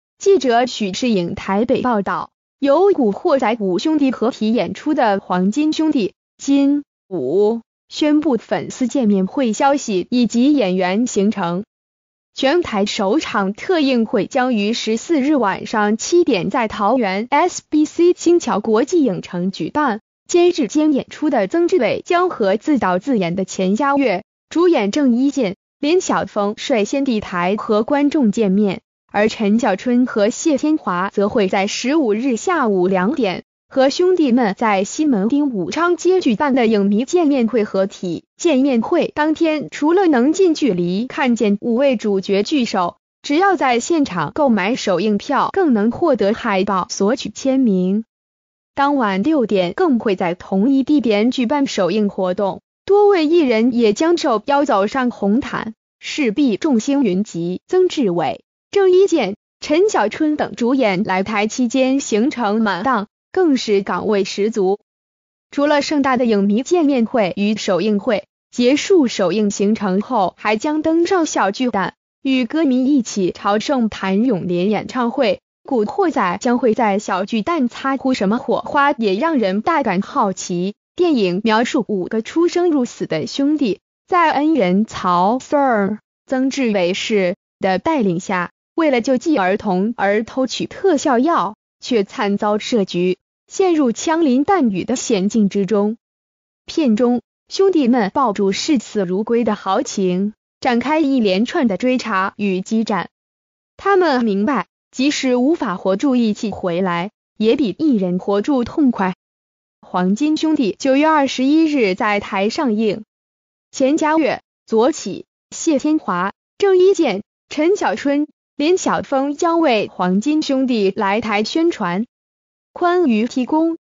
古惑仔台灣聚首 影迷朝聖港味行程看這裡！
〔記者許世穎／台北報導〕由「古惑仔」五兄弟合體演出的《黃金兄弟》，今（5）宣布粉絲見面會消息以及演員行程。全台首場特映會將於14日晚上7點在桃園「SBC星橋國際影城」舉辦，監製兼演出的曾志偉將和自導自演的錢嘉樂、主演鄭伊健、林曉峰率先抵台和觀眾見面，而陳小春和謝天華則會在15日下午兩點和兄弟們在西門町武昌街舉辦的影迷見面會合體。

發表評論評論如下。